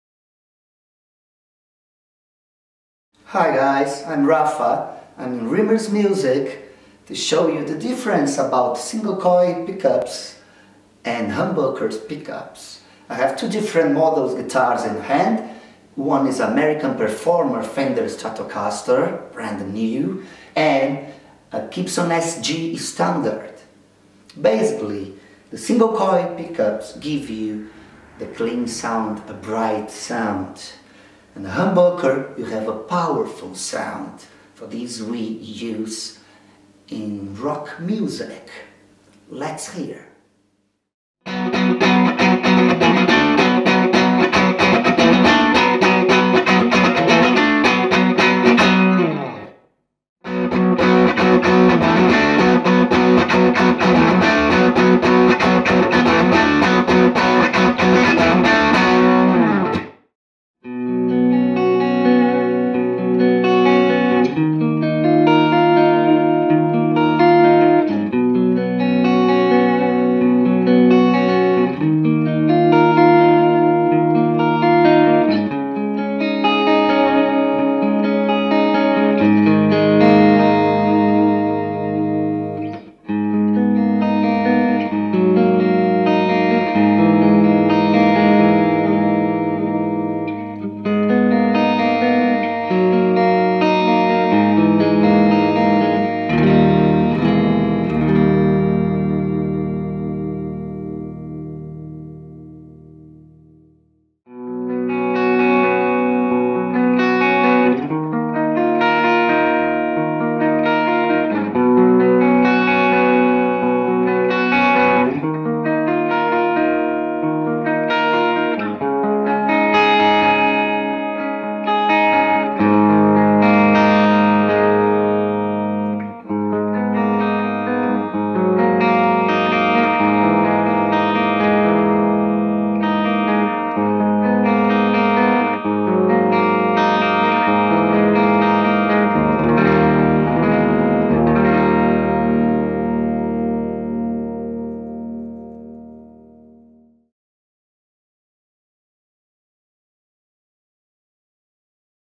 Humbucker Vs Single Coil Pick Ups! Rafa Raposo shows us the difference - Rimmers Music
Rafa Raposo shows us the difference between "Humbucker" and "Single Coil" picks and the benefits of each at our Bolton Store. Which do you prefer? Let us know...